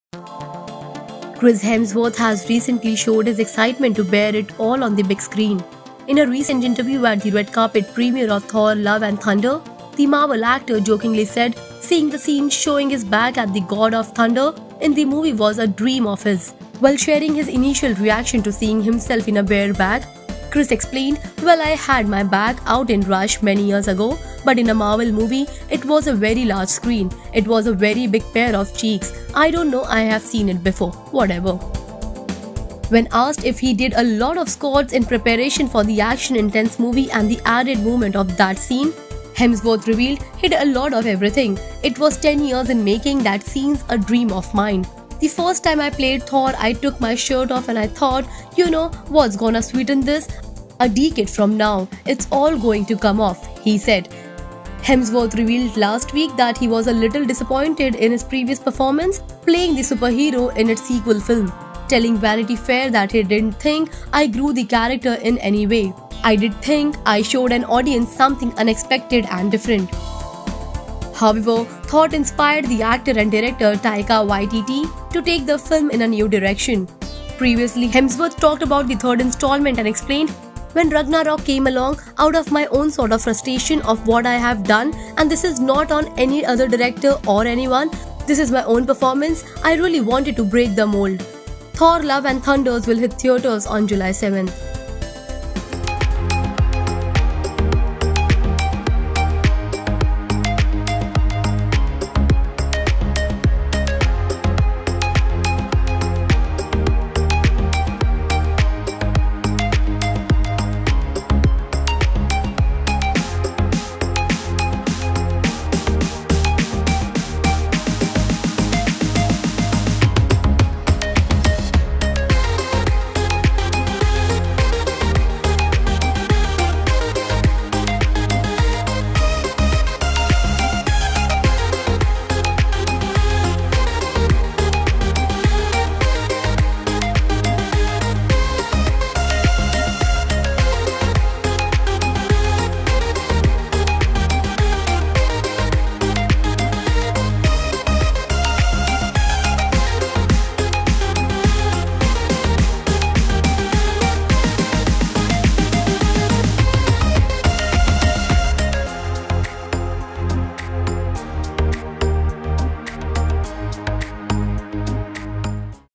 Chris Hemsworth Shares Deets Of Thor: Love And Thunder Semi-Naked Scene
Chris Hemsworth revealed last week that he was 'a little disappointed' in his previous performance playing the superhero in Thor sequel.